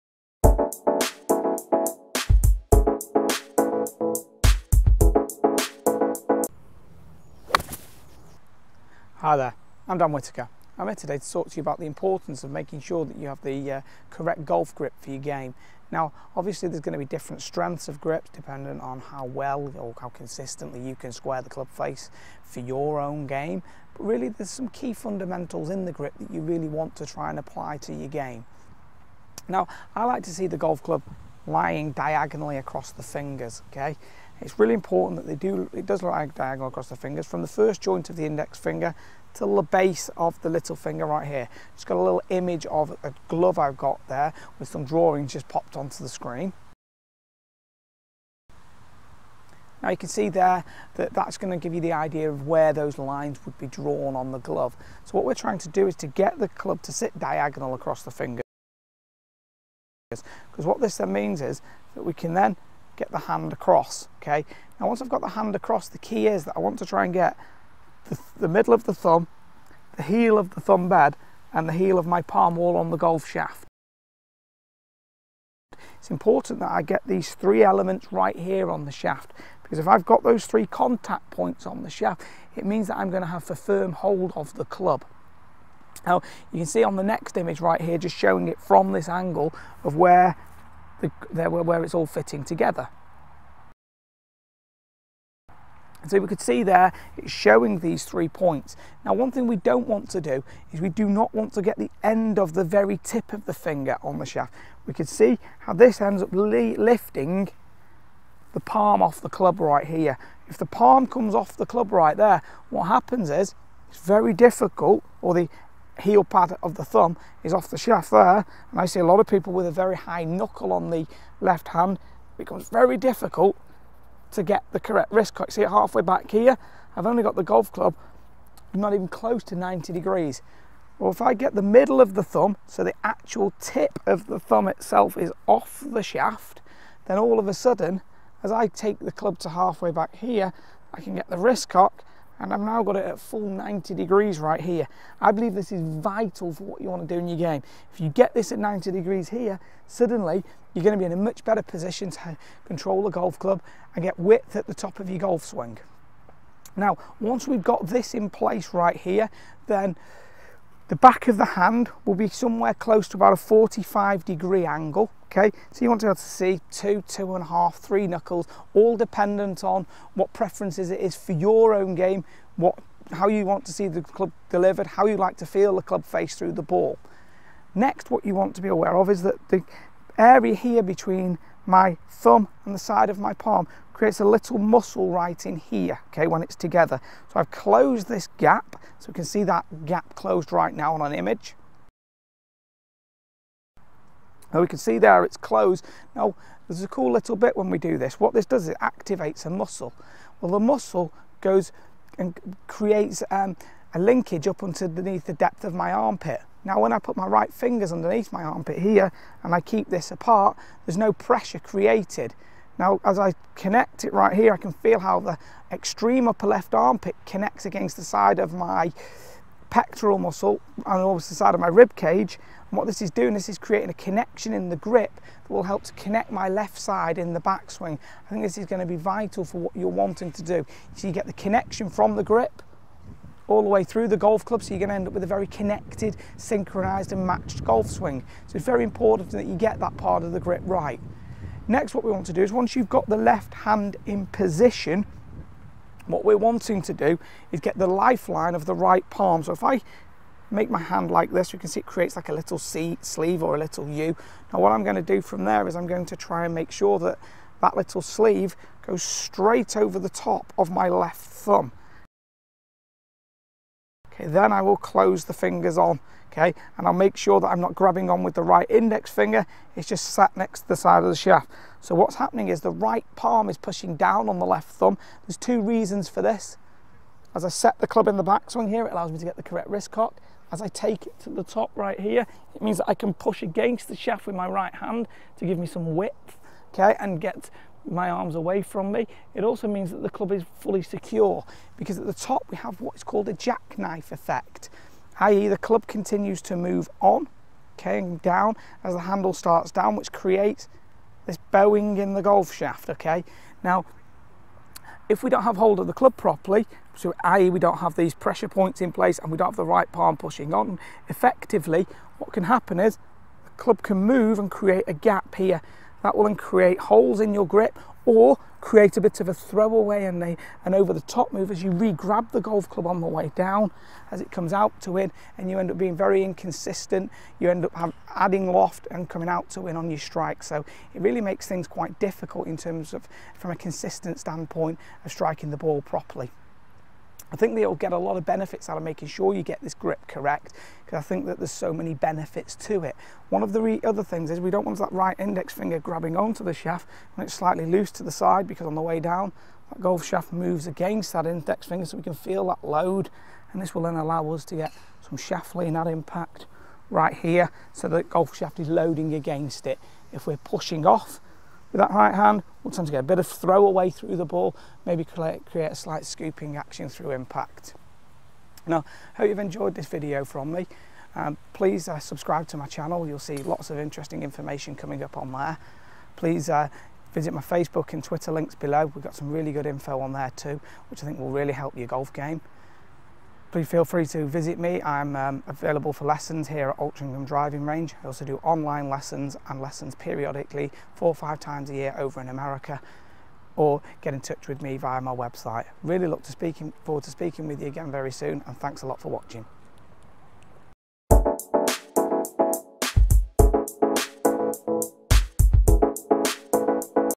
LEARN A GOOD GOLF GRIP FOR CONSISTENCY
Dan explains how a good golf grip can provide you with consistency in your game an how the hands fit together to control the golf club.